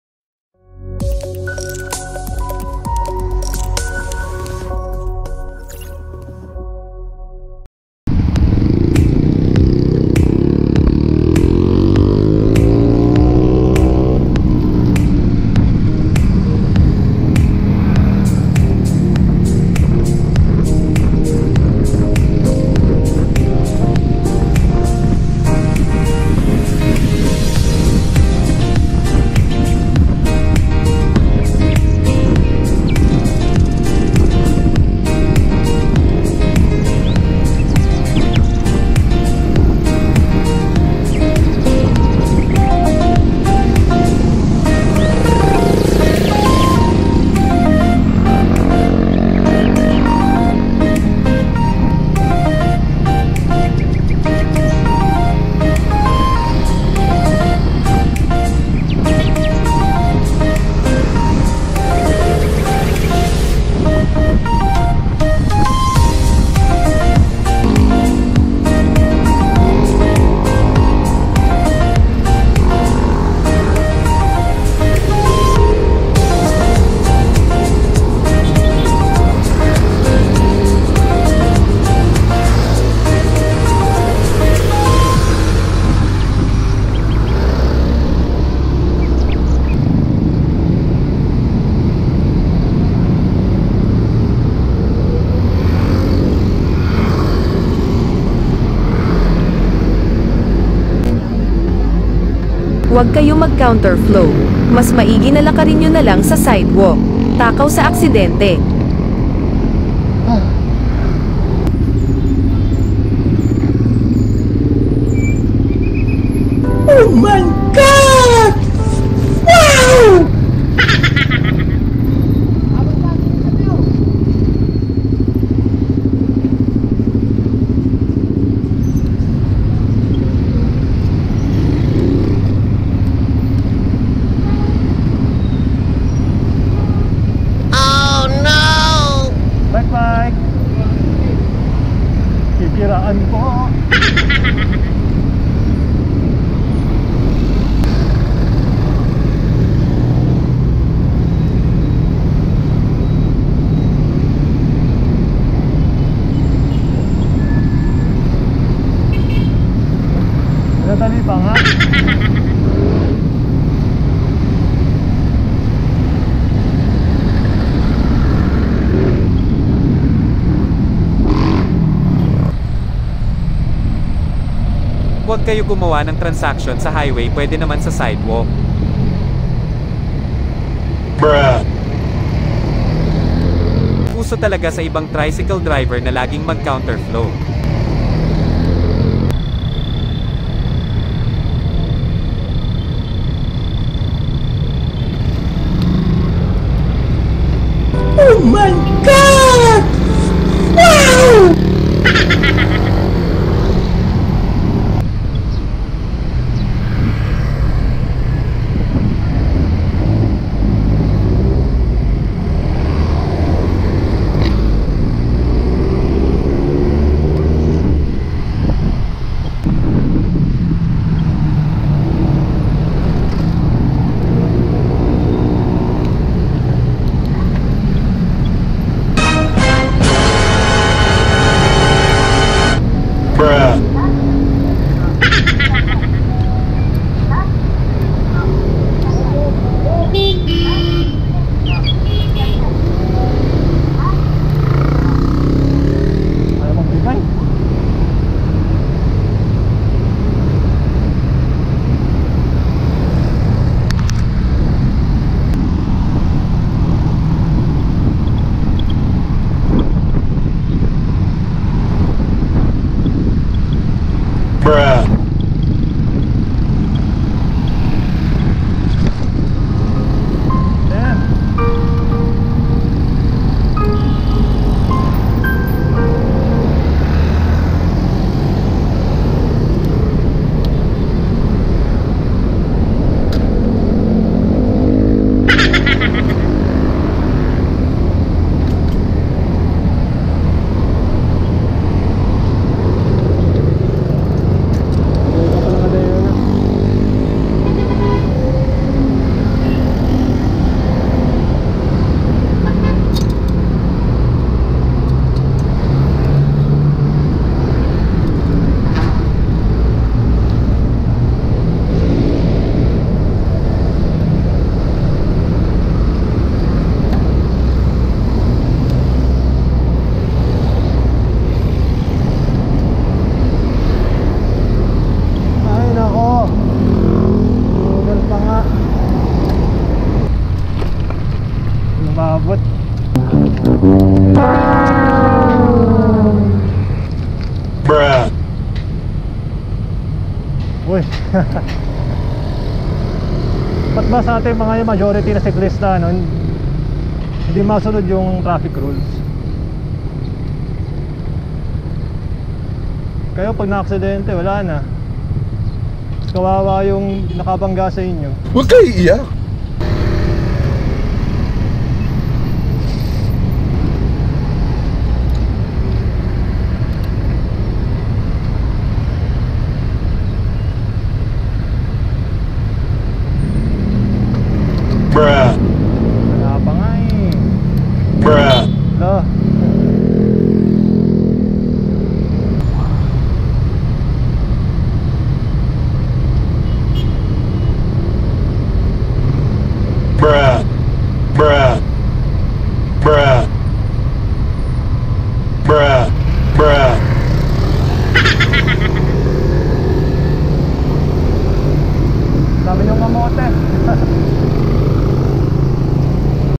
Share The Road - Jempoy and Kamote Compilation
Natapos nanaman ang isang linggo at ito na nga ang mga event na nakuha ko sa buong linggo. Enjoy guys kung kayo yung nakunan sa video wag niyo gagawin yung ginawa niyo napaka delikado.